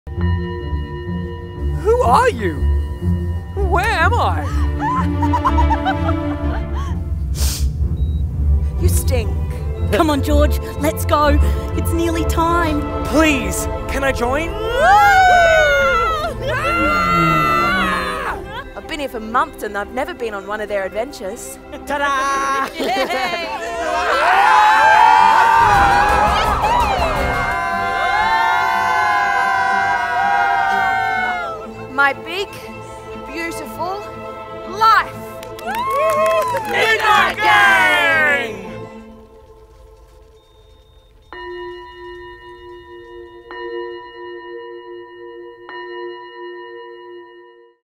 The Midnight Gang
Come on a magical adventure with the mighty Midnight Gang, as David Walliams best-selling novel comes to life on stage in a spell-binding production for one night only! Perfect for ages 6 -12 and their families.